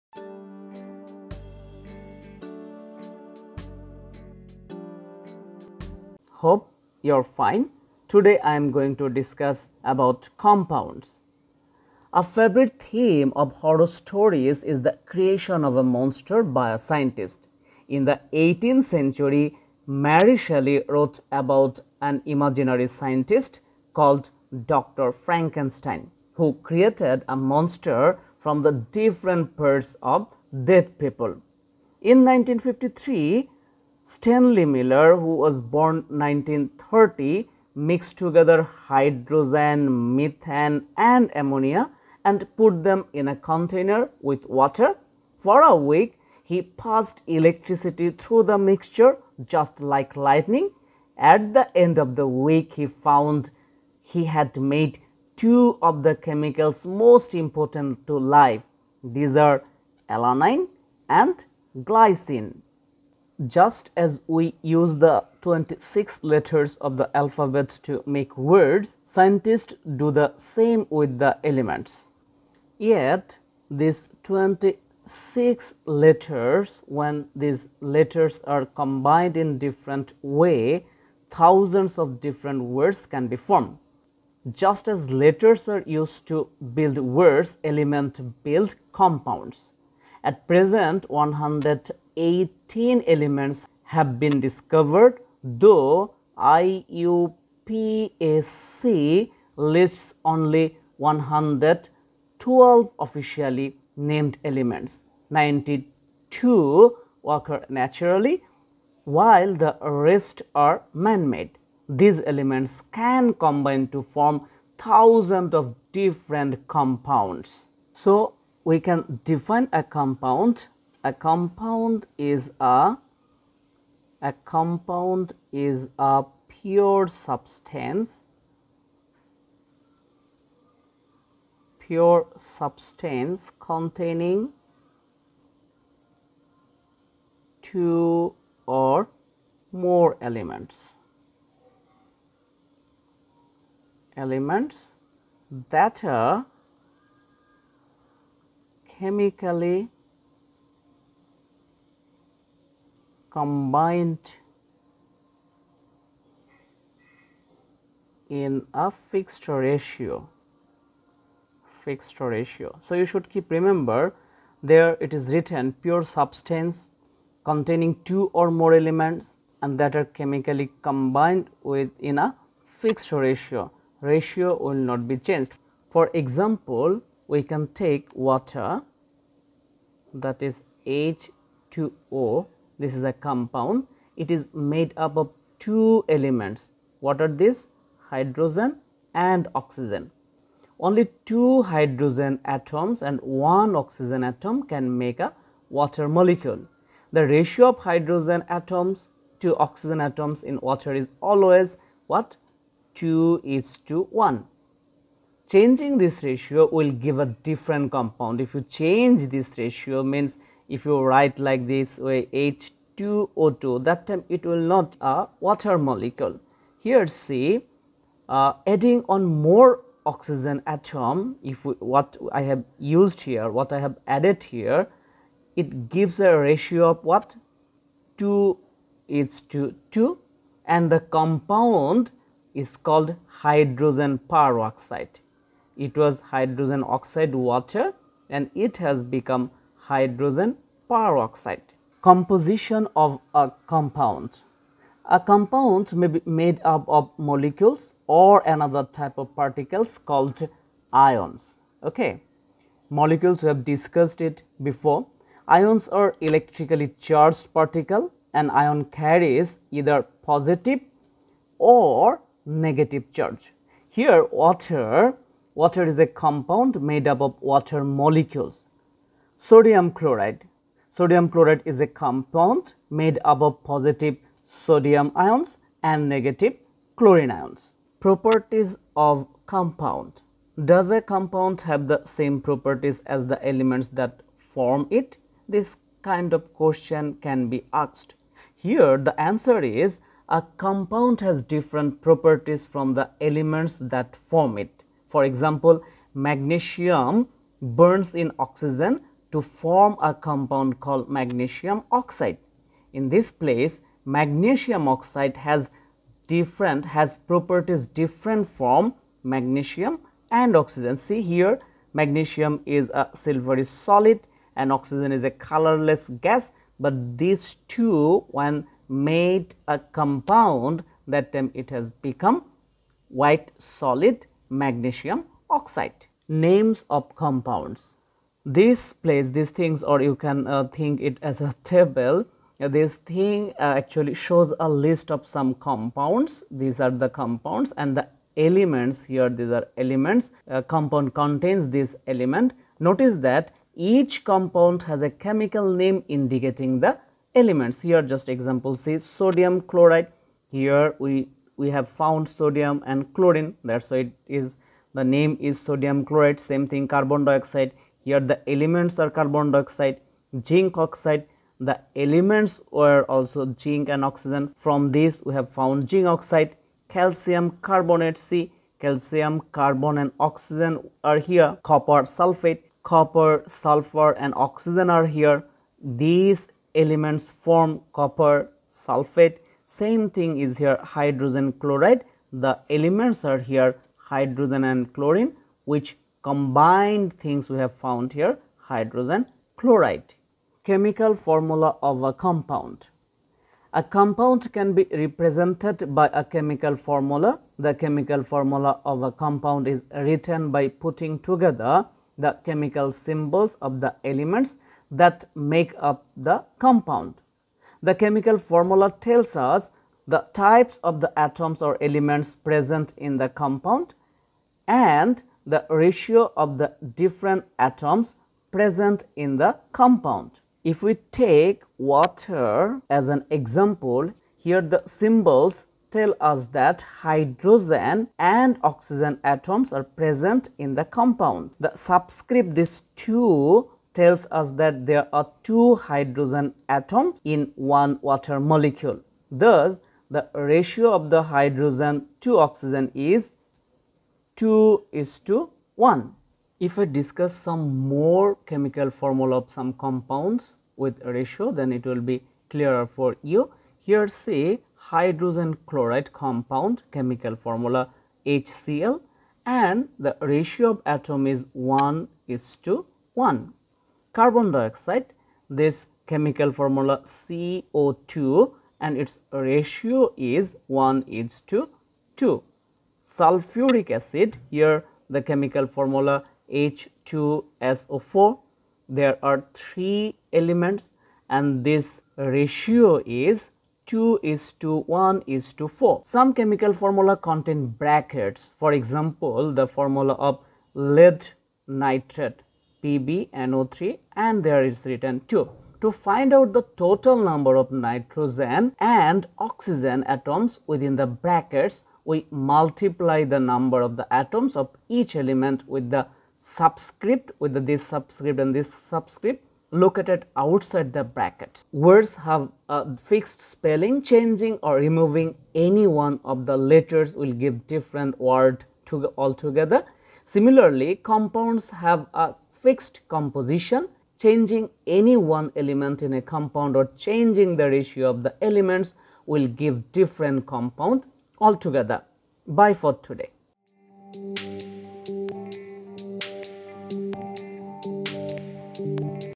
What is compound I compounds I compounds in chemistry I the empirical school
A compound is a pure substance which contains only one type of molecule made up of atoms of more than one element.